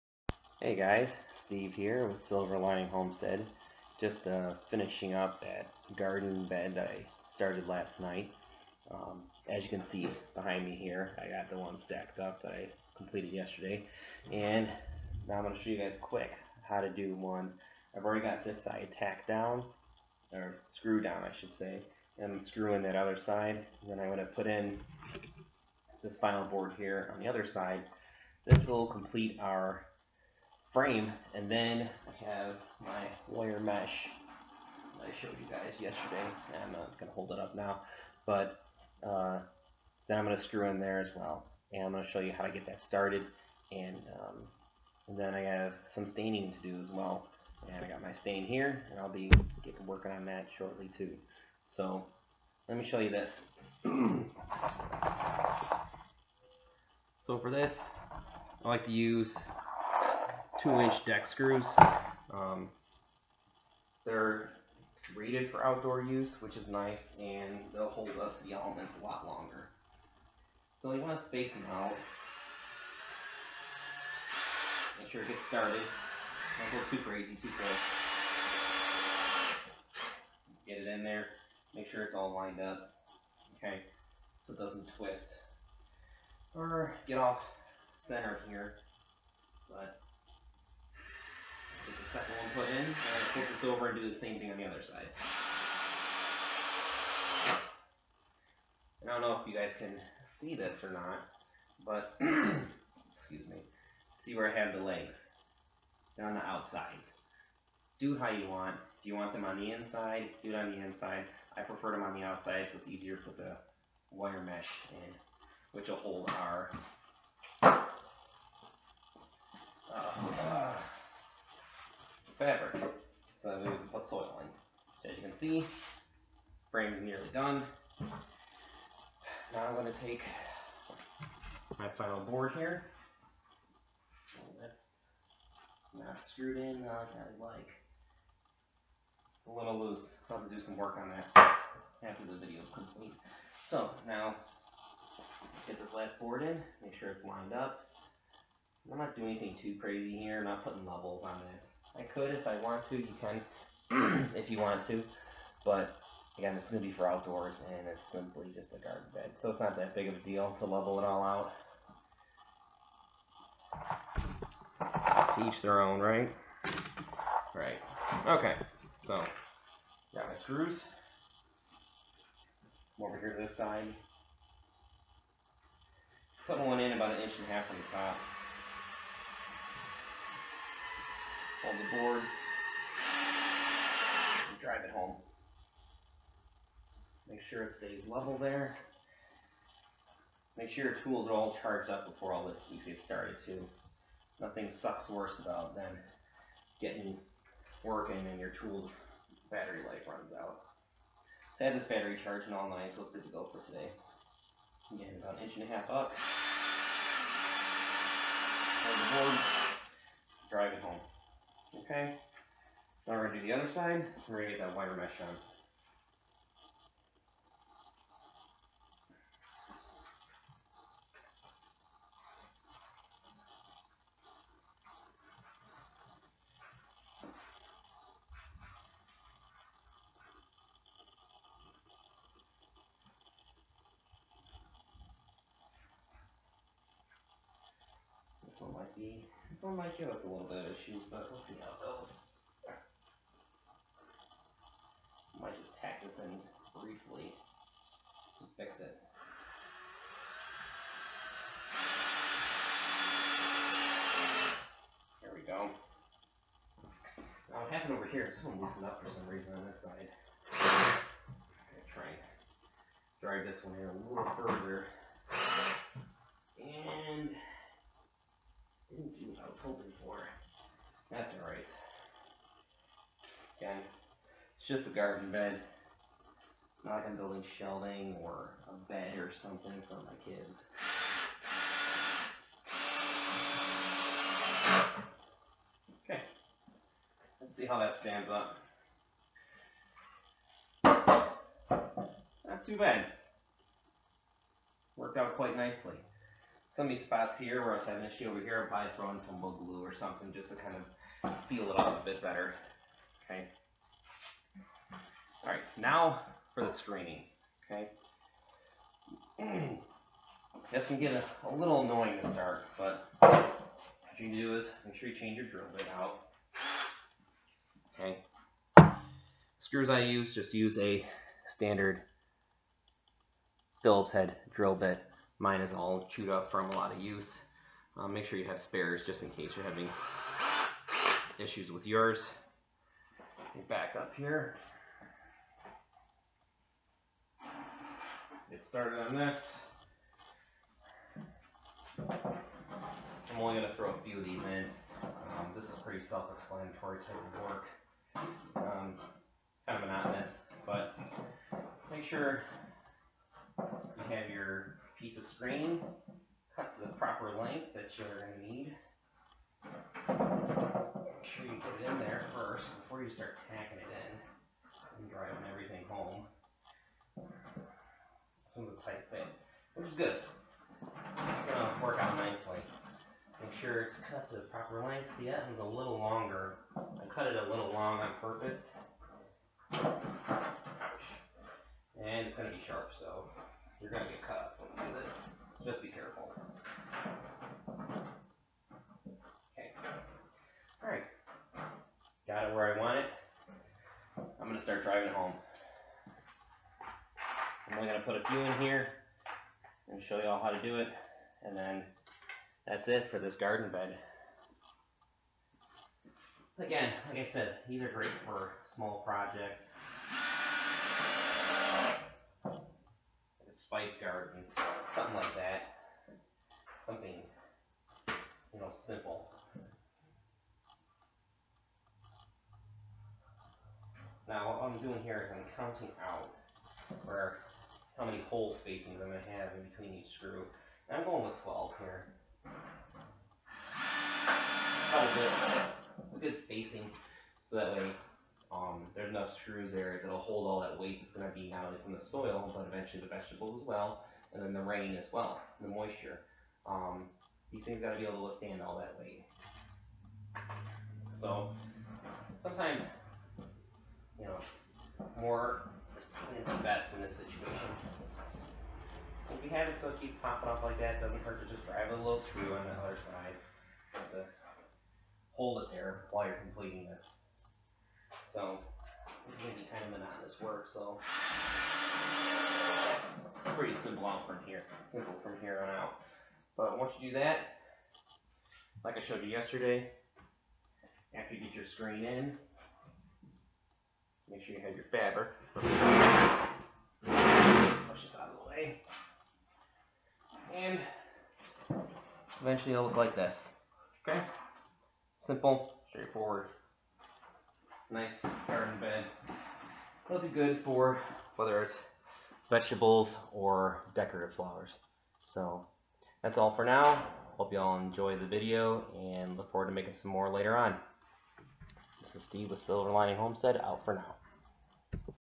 Finishing a raised garden bed.
Today I finished up the raised garden bed, all I need to do now is stain both of them. Now I will have three raised beds that will be great for smaller plants.